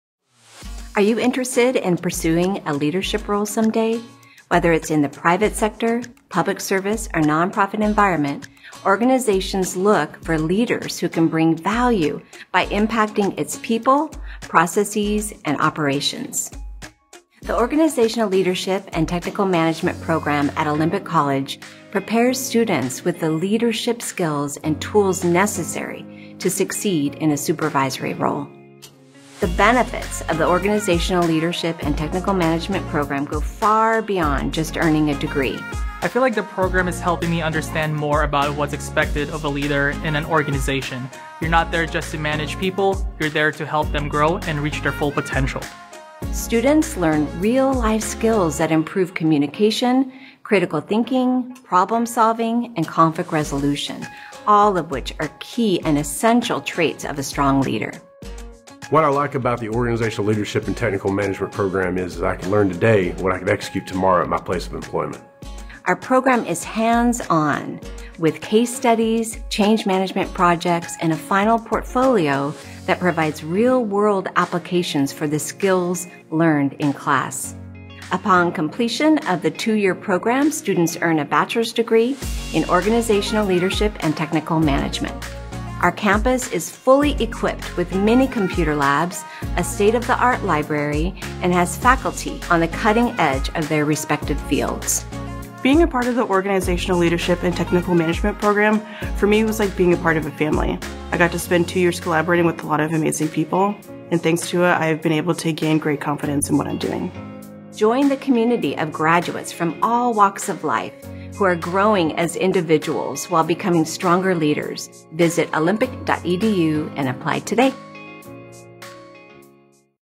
Study Organizational Leadership & Technical Management at Olympic College!
The Bachelor of Applied Science in Organizational Leadership & Technical Management at Olympic College will prepare you for leadership roles in private, public and nonprofit organizations. Students benefit from flexible class schedules, a state-of-the-art computer lab, and small class sizes allowing for individualized instruction.